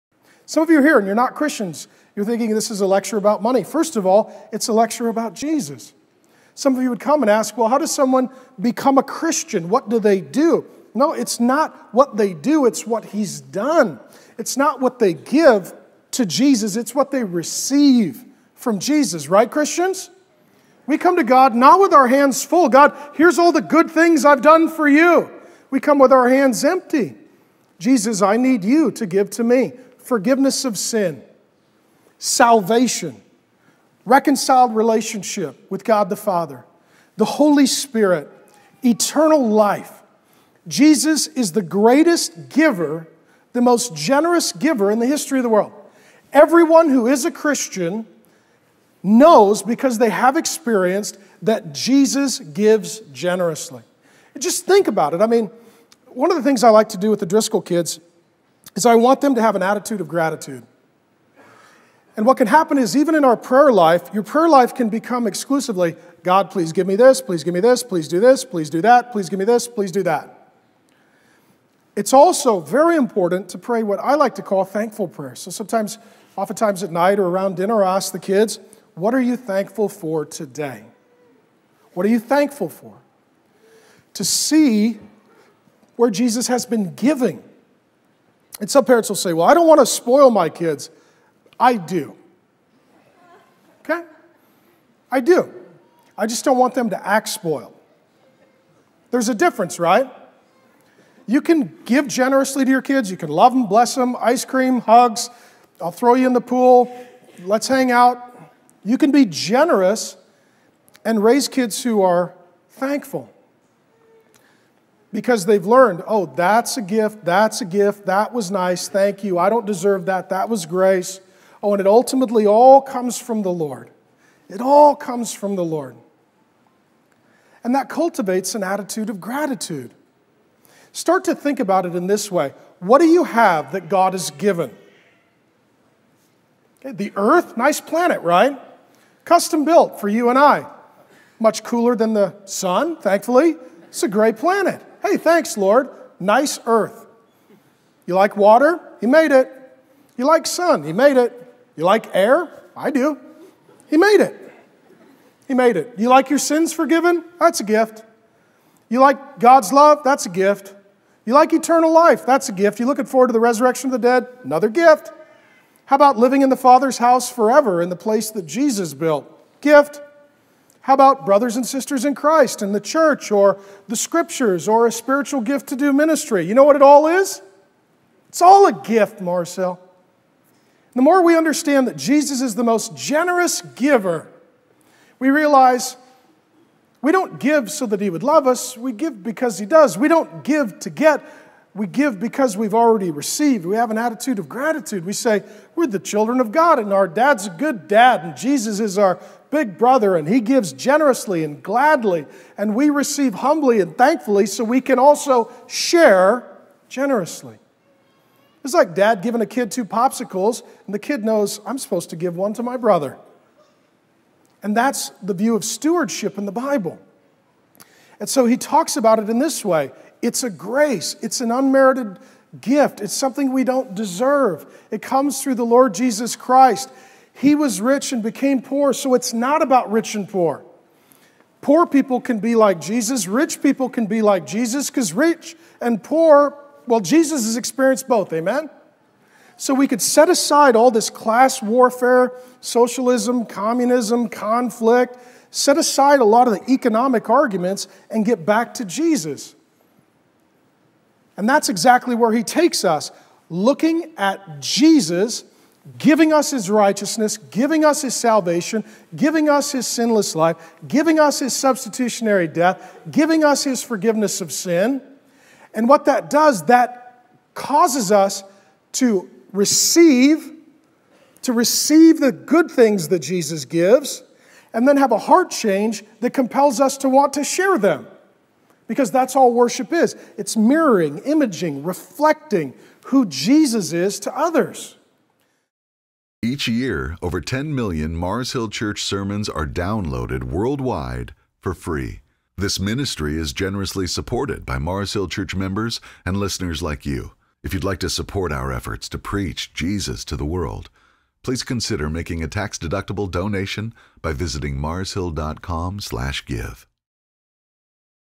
Jesus Is The Most Generous Giver Video
Jesus is the most generous giver ever: he was rich but became poor for us. Jesus sacrificed himself in our  lifes.